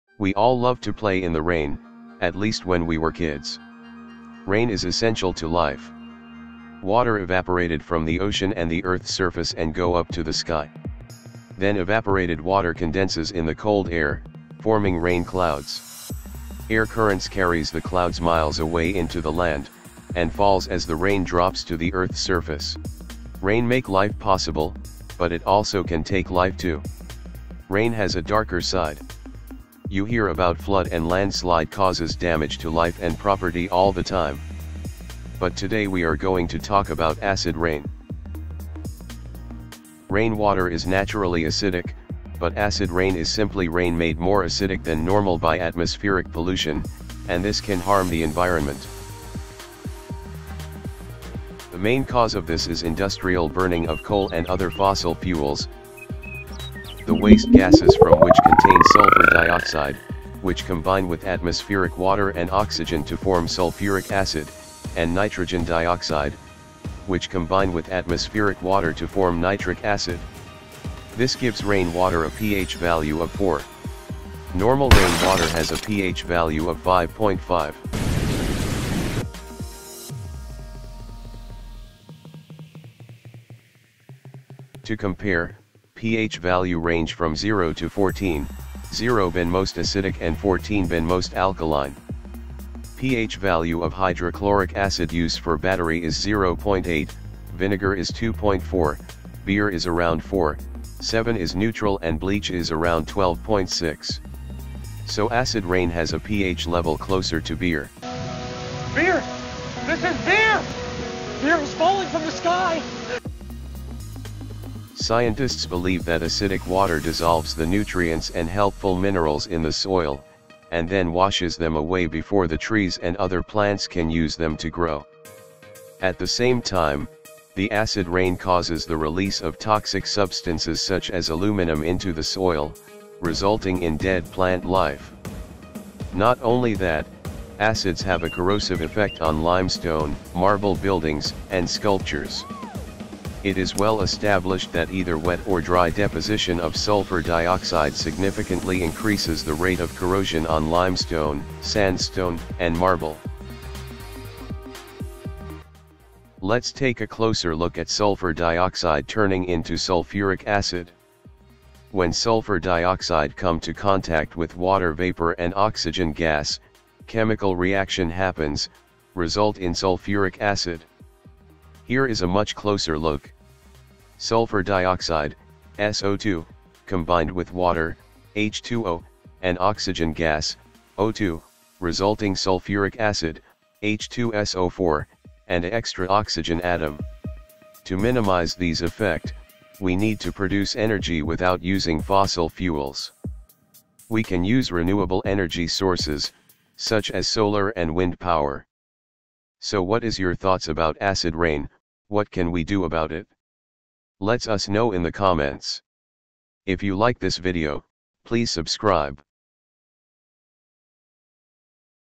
Let’s learn about Acid Rain
Rain water is naturally acidic, but acid rain Is simply rain made more acidic than normal by atmospheric pollution and this can harm the environment. The main cause is the industrial burning of coal and other fossil fuels, the waste gases from which contain sulfur dioxide, which combine with atmospheric water and oxygen to form sulfuric acid and nitrogen dioxide which combine with atmospheric water to form nitric acid. This gives rain water a pH value of 4. Normal rain water has a PH value of 5.5.